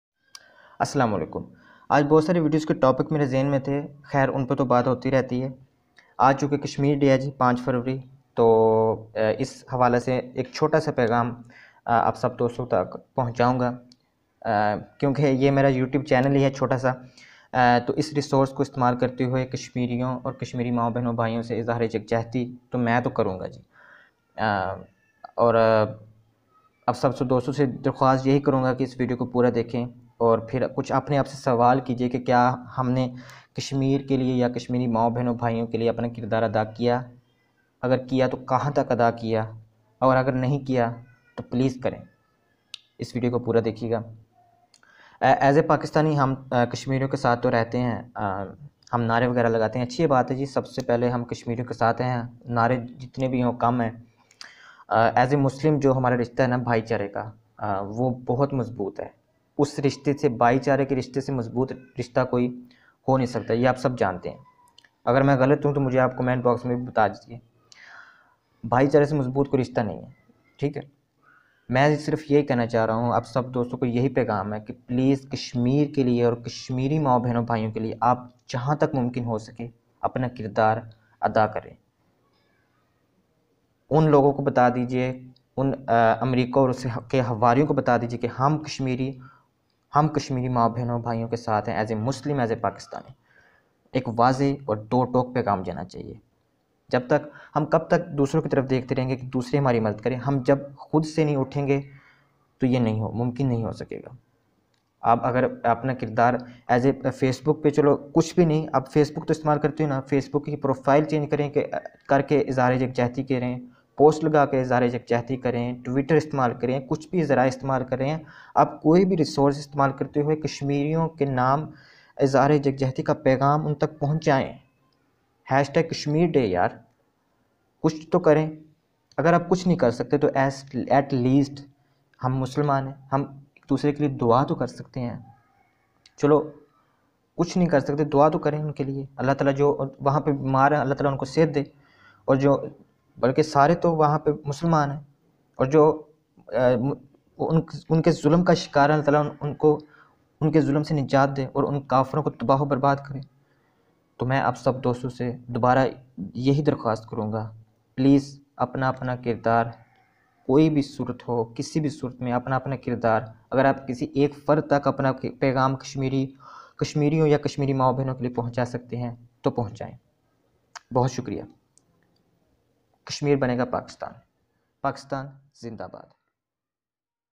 کشمیر ڈے پر میرا پیغام😢
پلیز  اپنا اپنا کردار لازمی طور پر ادا کریں۔ ہم کشمیر اور کشمیری ماوں،بہنو اور بھائیوں کے ساتھ ہیں۔

Kashmir day 5th February.

 کشمیر بنے گا پاکستان انشااللہ۔